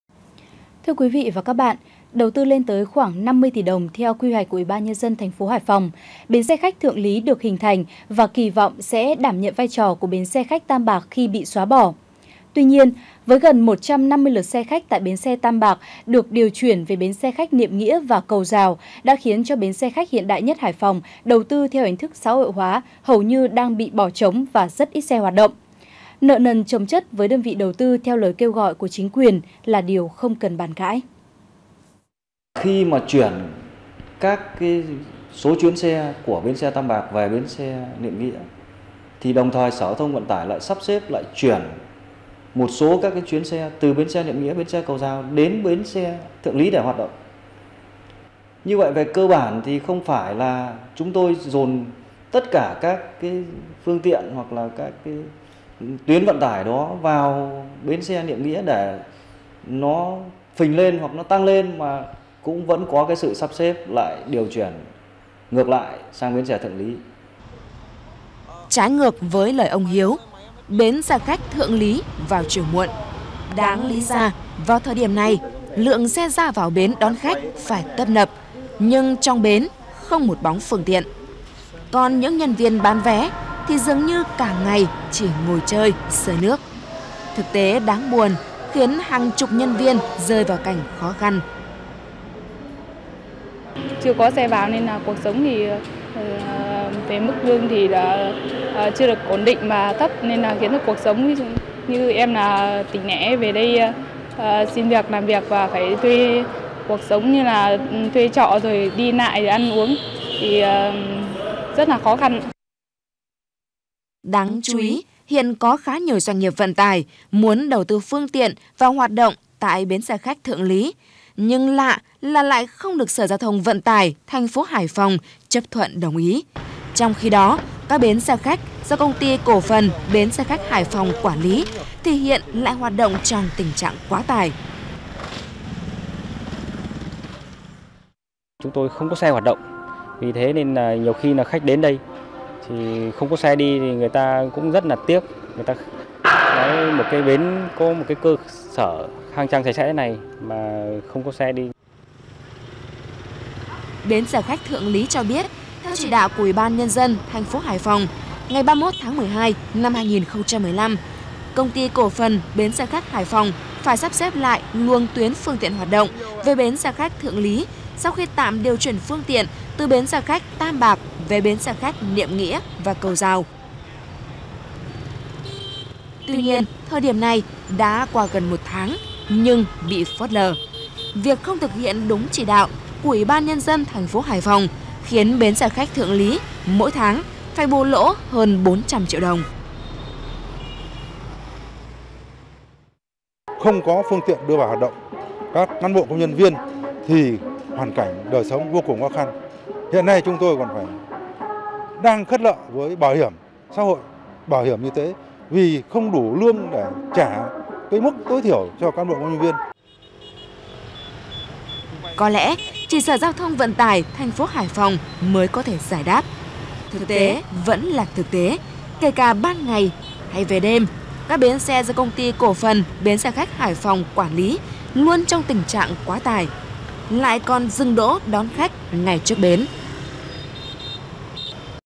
(VTC14)_Hải Phòng : Vì sao bến xe 50 tỷ kêu cứu?
(VTC14) - Đầu tư lên tới khoảng 50 tỷ đồng theo quy hoạch của UBND TP Hải Phòng, Bến xe khách Thượng Lý được hình thành và kỳ vọng sẽ đảm nhận vai trò của Bến xe khách Tam Bạc khi bị xóa bỏ. Tuy nhiên, với gần 150 lượt xe khách tại Bến xe Tam Bạc lại điều chuyển về Bến xe khách Niệm Nghĩa và Cầu Rào đã khiến cho bến xe khách hiện đại nhất Hải Phòng đầu tư theo hình thức xã hội hóa hầu như bỏ trống, rất ít xe hoạt động. Nợ nần chống chất với đơn vị đầu tư theo lời kêu gọi của chính quyền là điều không cần bàn cãi.